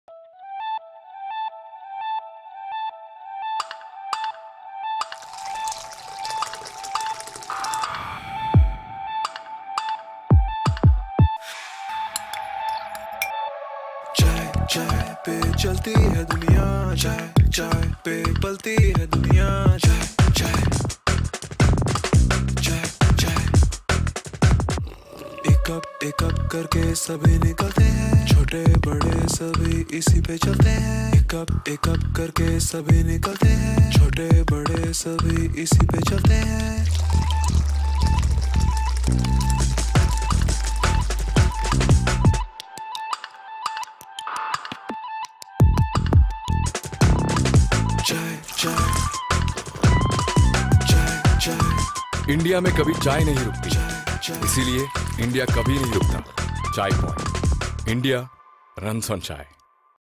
India Runs On Chai
Our tribute to the drink that fuels the nation and keeps it alive awake and progressive.